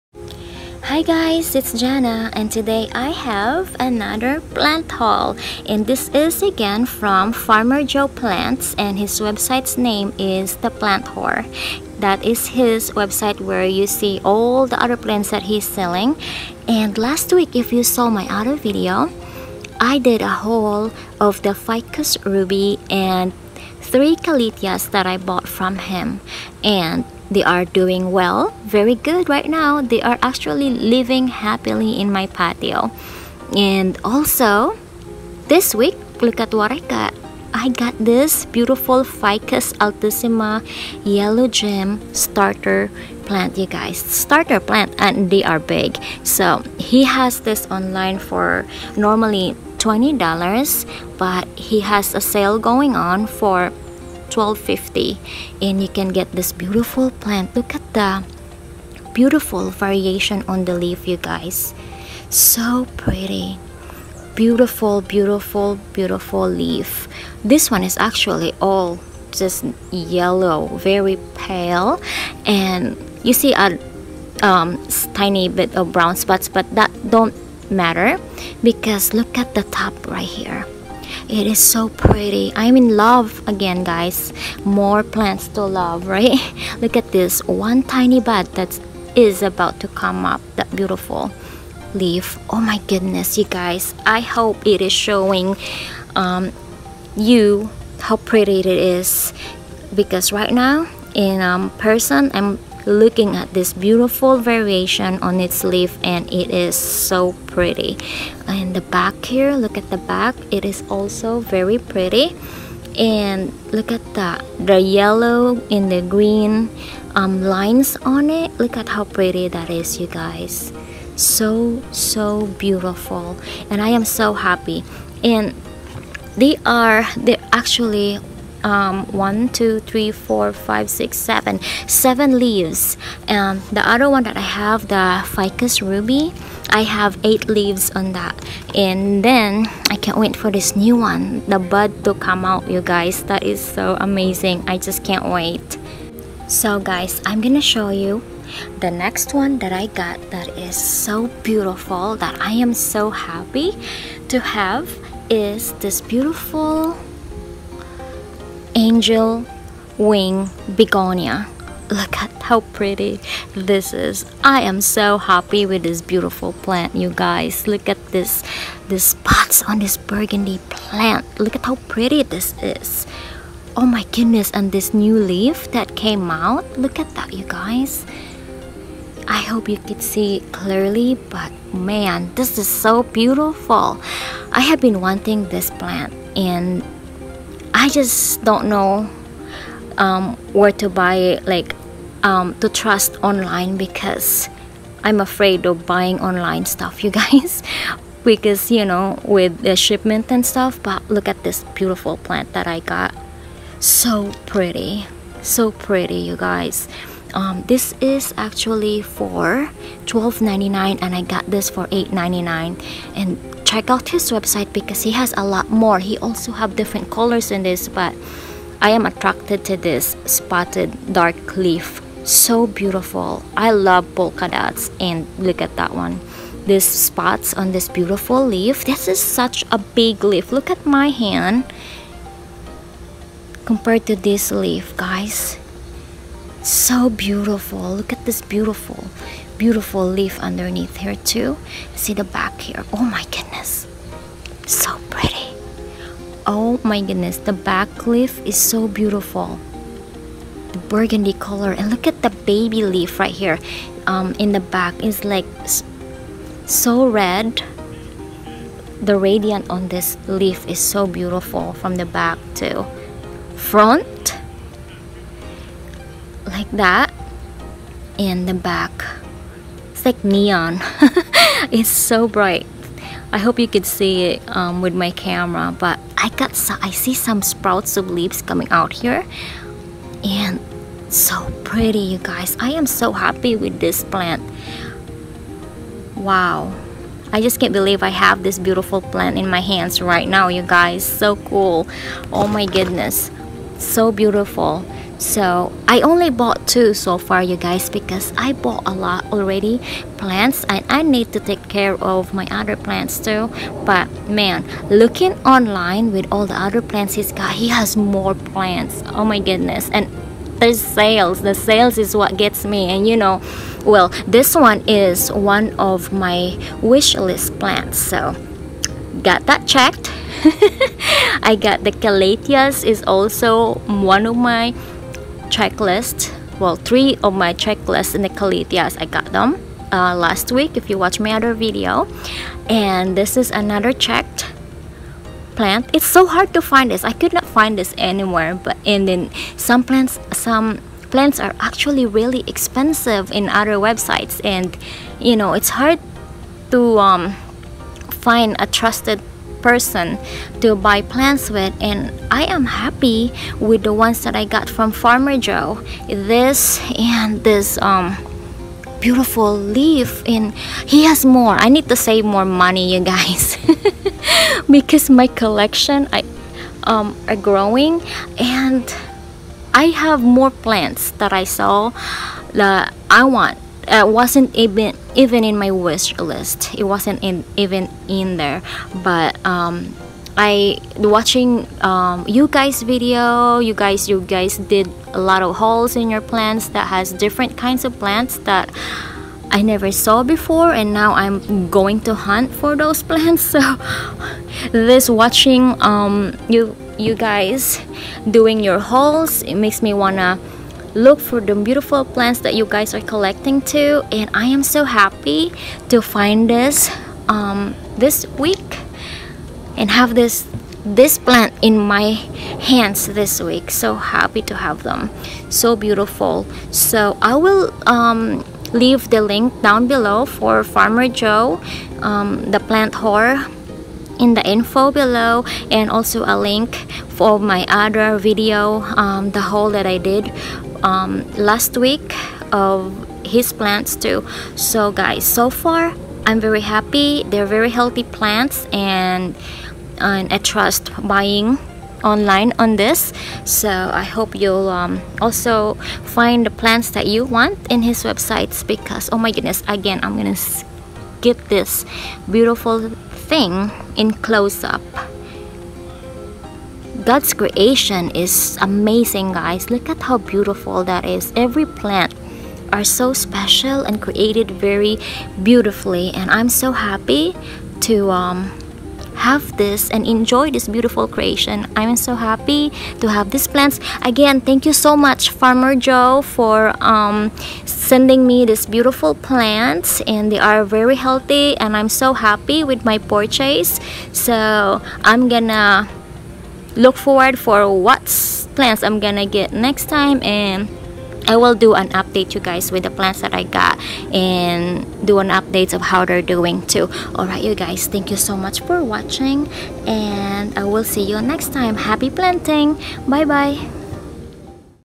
Begonia Angel Wing and Ficus Altissima / Farmer Joe Plants
Hello friends! I have another plant haul from Farmer Joe Plants. They are so cool looking and I'm so happy I got them, so glad to have my wishlist plant on hand.